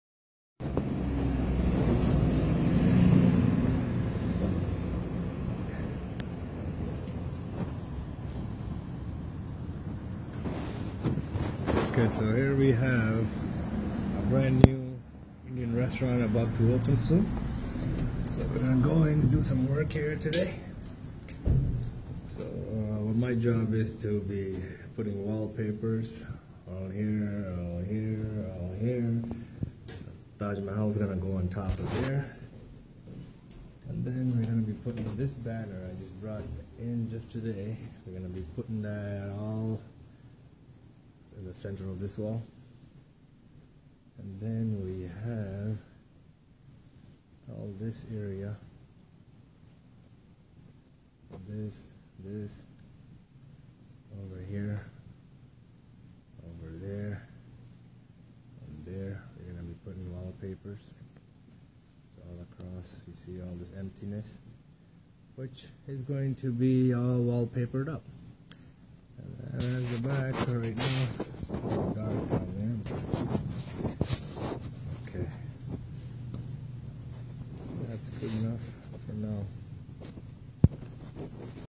Wallpaper Install For Restaurant Vid 1/2
Wallpaper Install..a look at what the wallpaper looks like..it is a puzzle that needed to be solved. a view of the place first...before video..